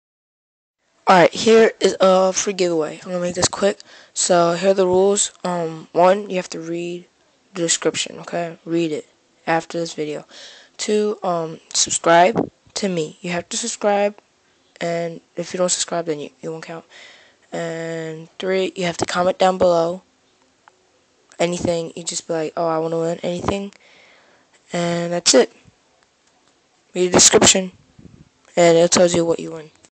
Free Giveaway!
2. Comment
3. There is only one winner
4. the winner will get...
-some pivot cups
-five random blackriver or flatface stickers
-any decks of their choice. I have a four corner hc mold and the mellow mokd that r refinished with paint, or a wooden cruiser. Legit things.
I will end the giveaway when I have 50 subscribers or more then 10 people entering.